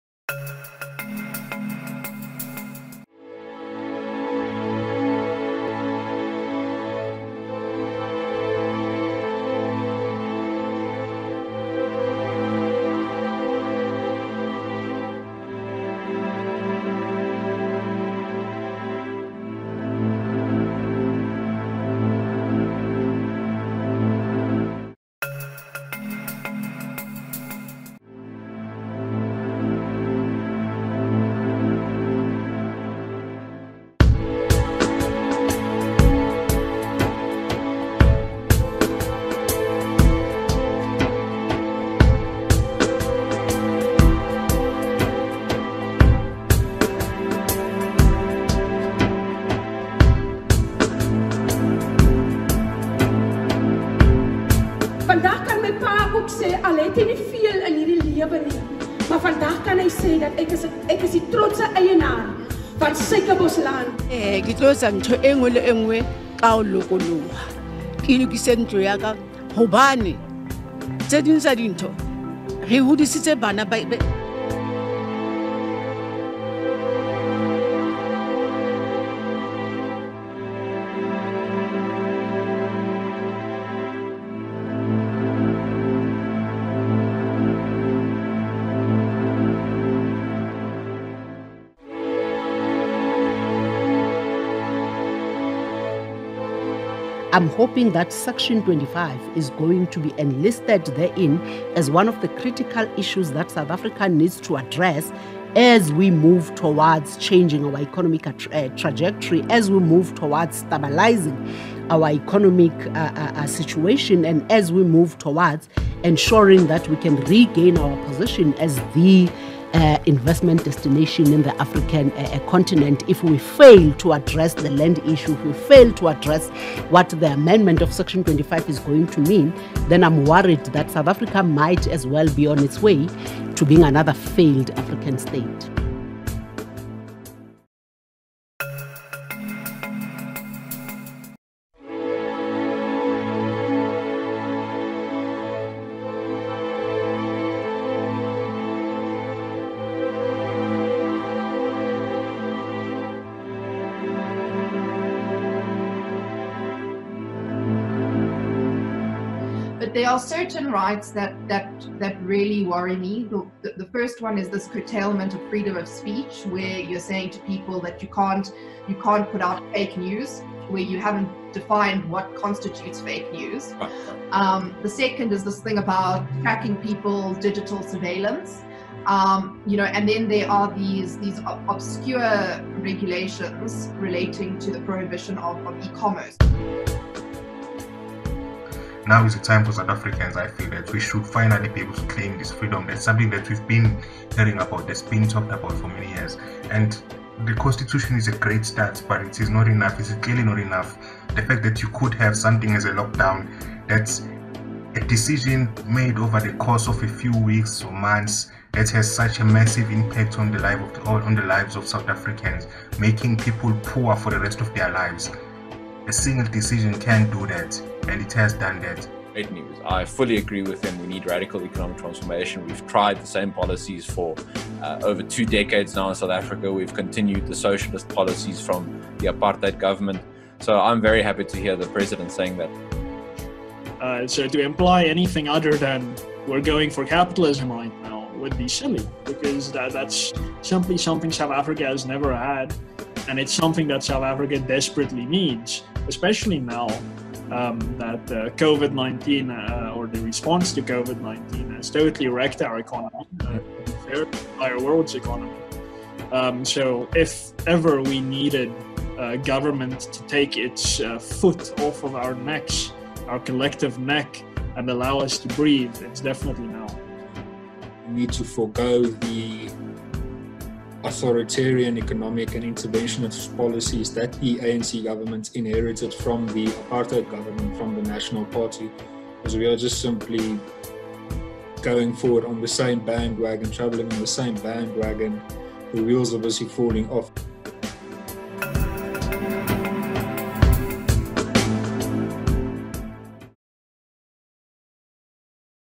FMF showcase - Atlas Network Liberty Forum & Dinner
Atlas Network* hosted its annual Liberty Forum & Freedom Dinner virtually this year, and made provision for partners to showcase their work "in front of an audience of international peers and donors" in their virtual exhibitor hall. This is the FMF's contribution.

*Atlas Network increases global prosperity by strengthening a network of independent partner organizations that promote individual freedom and are committed to identifying and removing barriers to human flourishing.